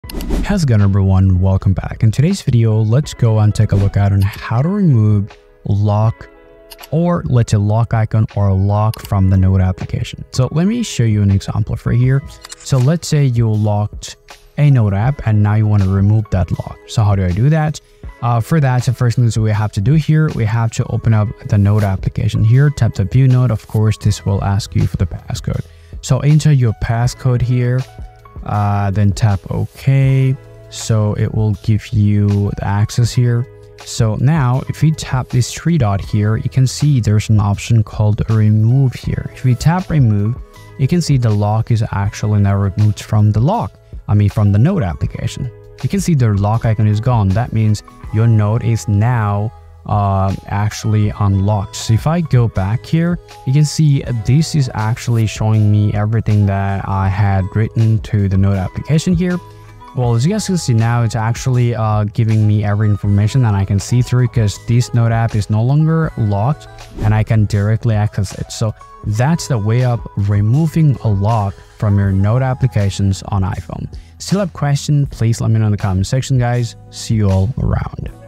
How to Remove Lock from Notes on iPhone | Unlock and Access Locked Notes
Want to remove the lock from a note on your iPhone? I’ll show you how to easily unlock your notes and remove the password protection, so you can access them quickly without any hassle. Removing a lock from your notes can be useful if you no longer need extra security on certain notes and want quicker access.

Follow along as I guide you through the steps to unlock and disable the password for your notes. By the end, you’ll know how to remove locks from your iPhone notes with ease!